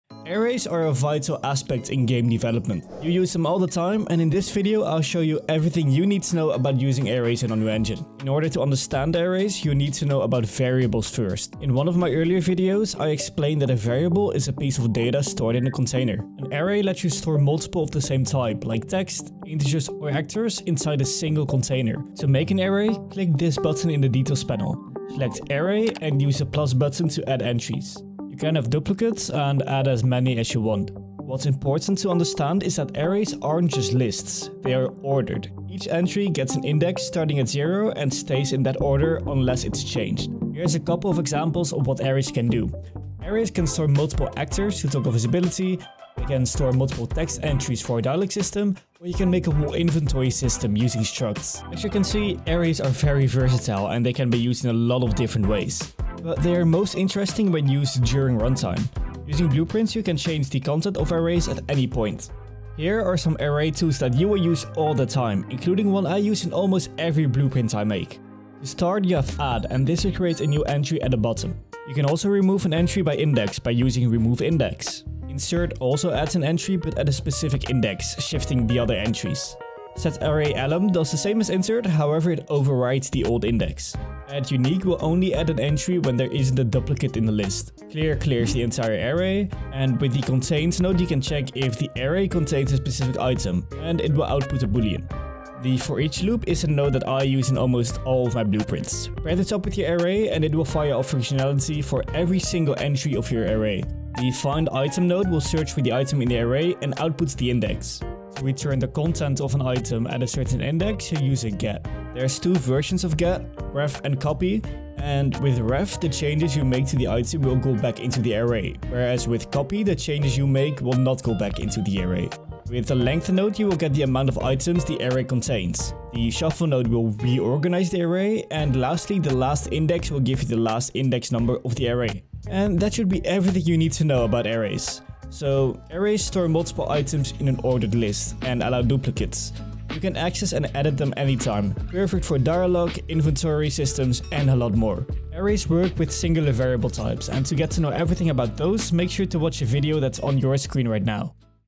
Master ARRAYS in 2.5 minutes!
In this video I'll quickly explain what you need to know about arrays. There are several blueprint nodes that you can use with your arrays. I go over the most important ones in this video.

0:00 - Intro
0:12 - What is an array?
1:09 - Array tools
2:16 - Summary